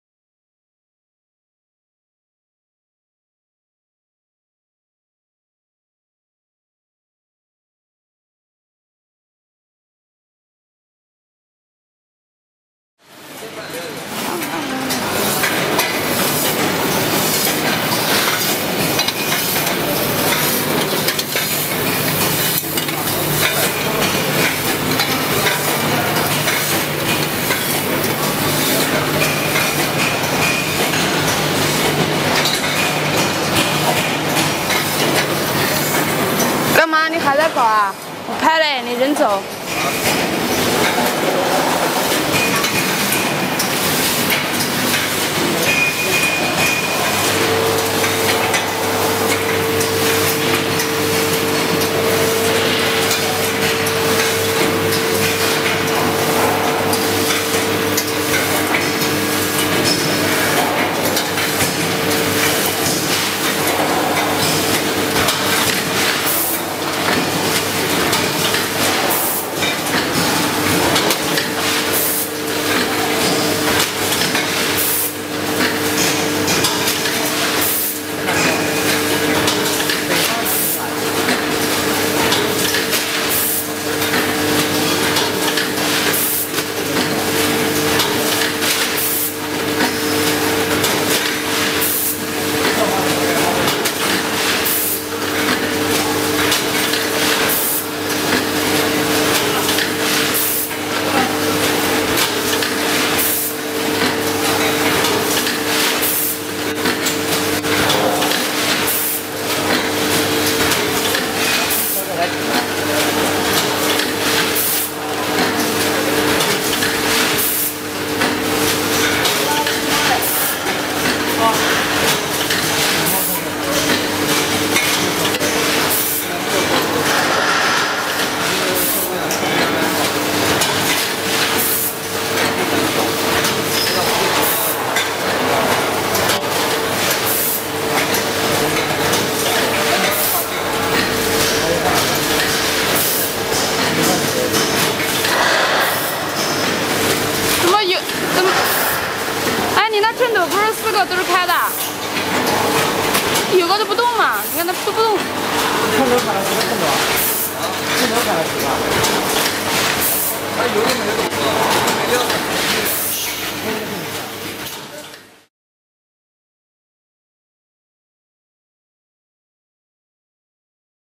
VFS7300 Granule Packing Machine—AnHui ZengRan
This machine consists of CJSL2000 auger feeder and one VFS7300 verical packaging machine,the special metering method own national patent.

Address:No. 65 Jiqiao Rd.,Luyang Industrial Park, Fuyang Ave, Hefei, Anhui, China;